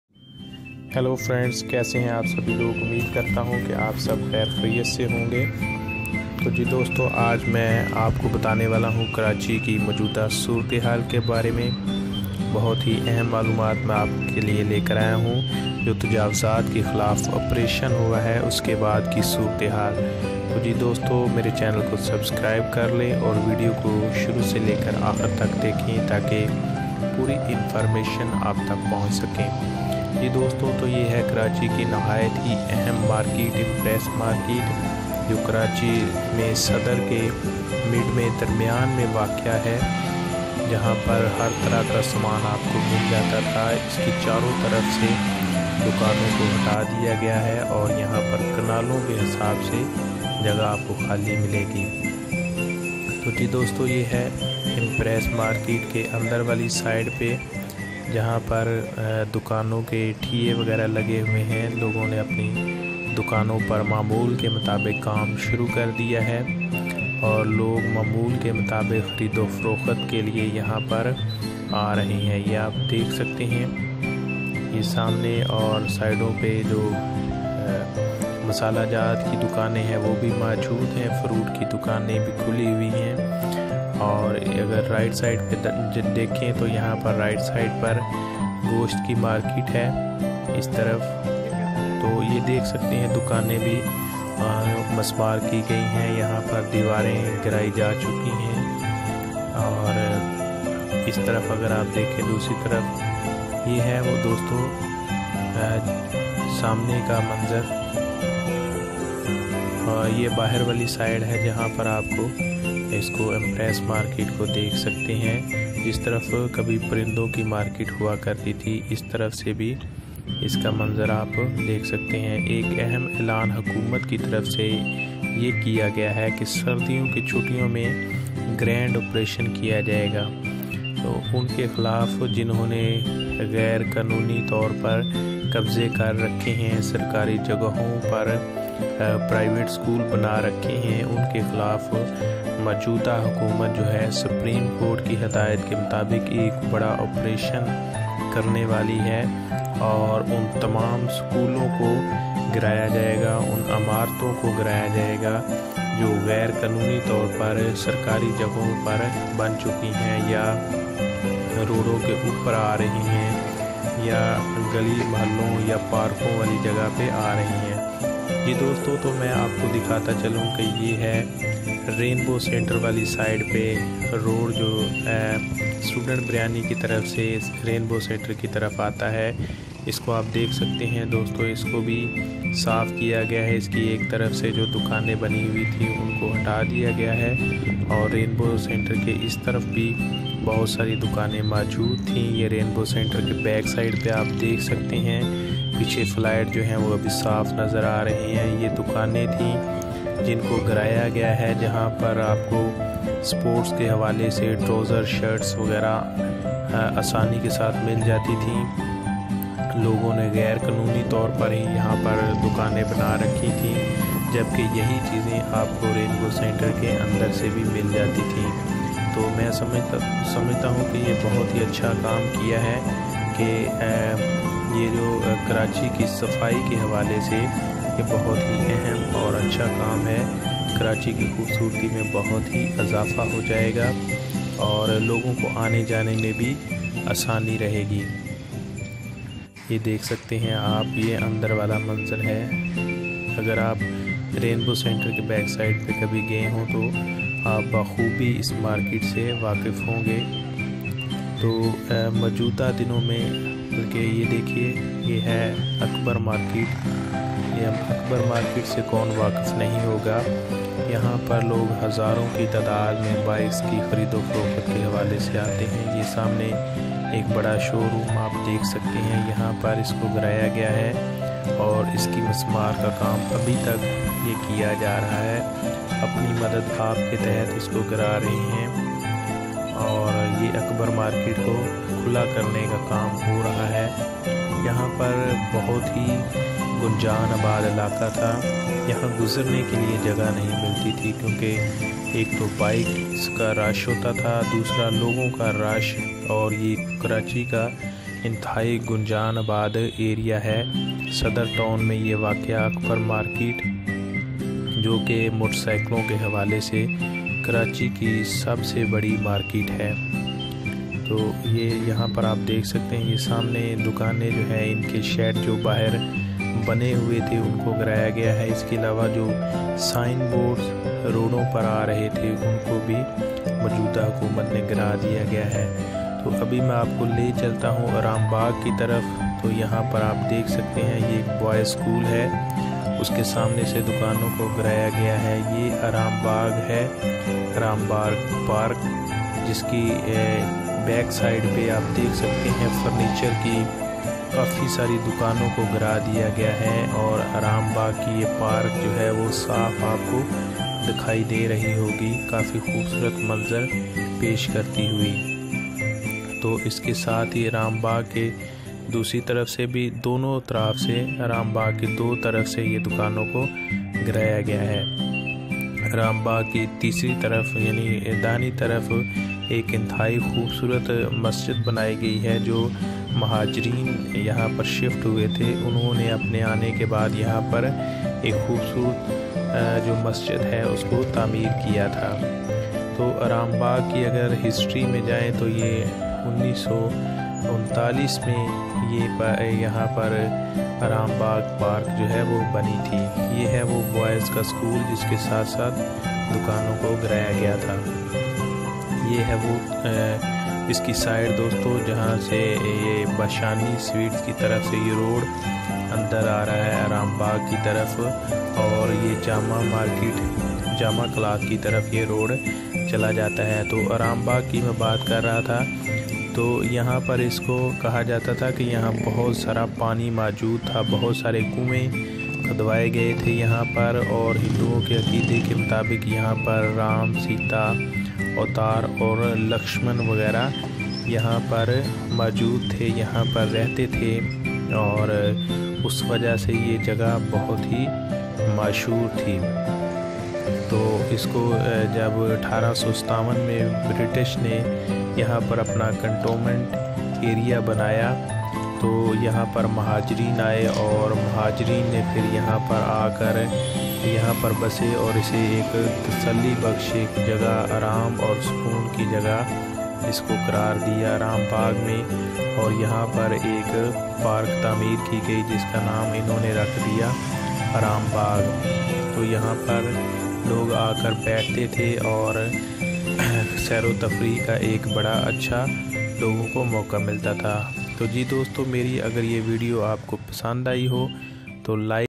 Saddar Town Karachi and neighboring areas after anti encroachment operation.
Nice video
Atrium Mall saddar,
It is Tops&tech YouTube channel.
This video guide you about the condition of Karachi in present days. Empress Market saddar, rainbow center, Akbar Market saddar and Arambagh,these areas are conclude in this video. So I want to tell you that condition of these areas are now satisfactory after anti encroachment operation. Many shops are opened up and business of people on its way.
I shares videos on YouTube regarding top news and technology related.